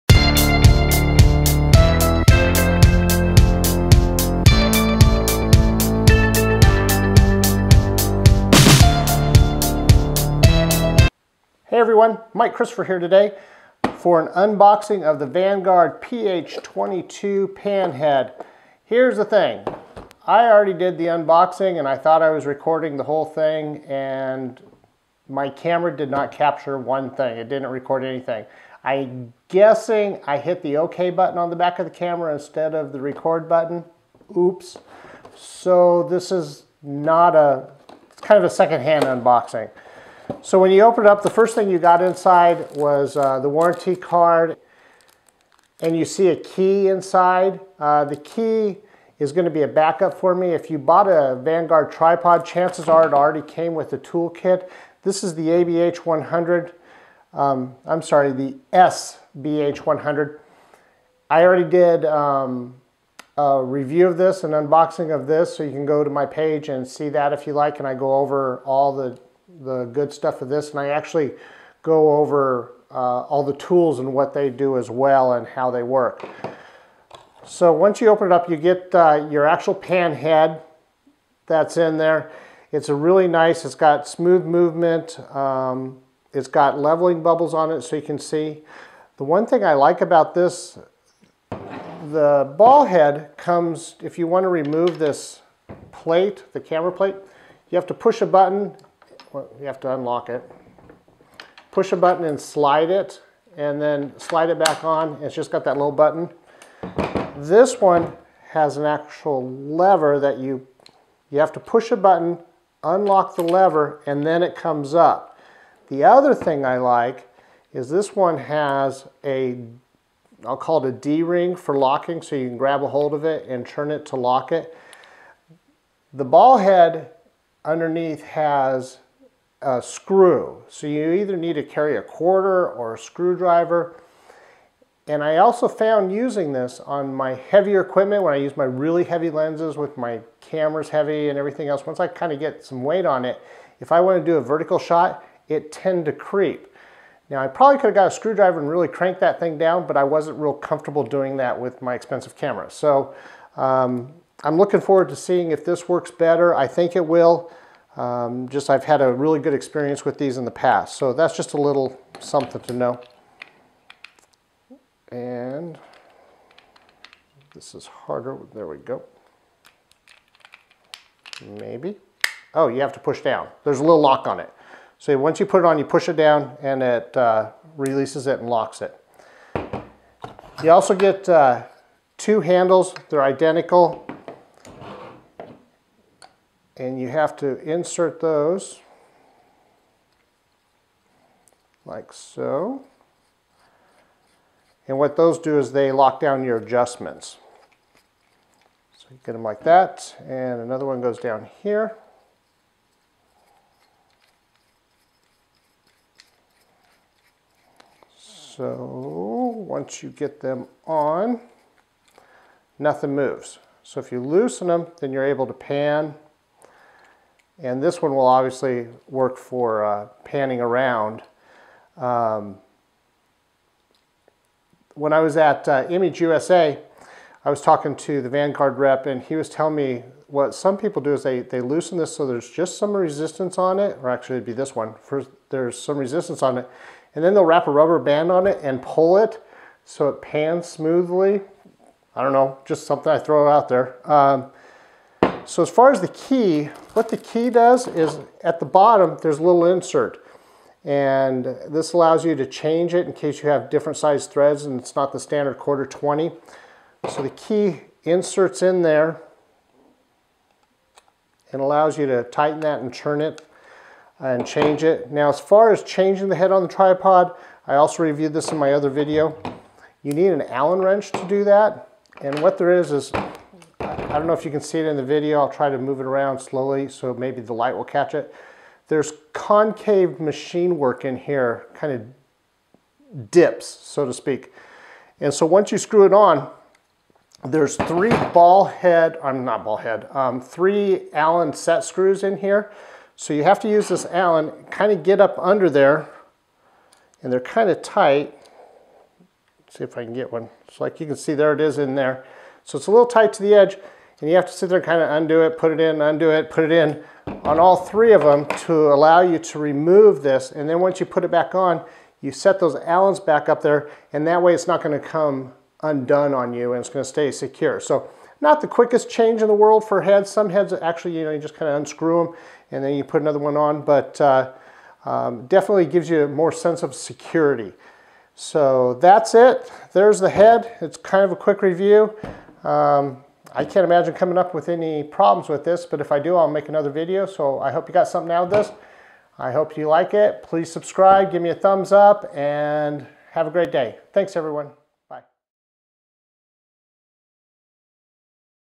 Vanguard PH-22 Tripod Pan Head - Unboxing
Hey everyone, this is a quick look at the Vanguard PH-22 Magnesium Alloy Pan Head - Tripod head!